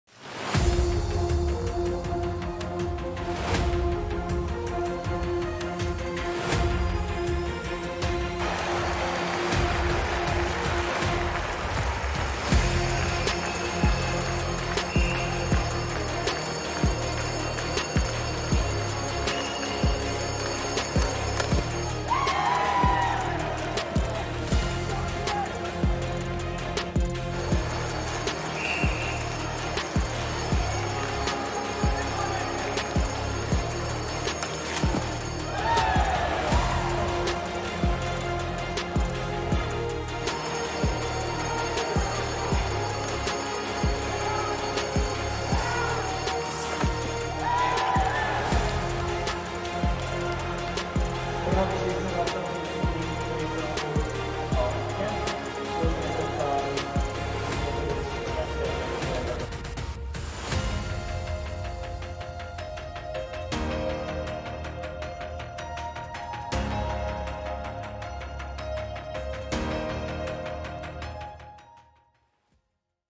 09.10.20 - L'action de la semaine: les arrêts décisifs
Lors du comeback des Grenat face aux ZSC Lions vendredi 09.10.20, Gauthier Descloux a dégouté les attaquants adverses durant la séance de tirs au but. Le tout avec des arrêts spectaculaires!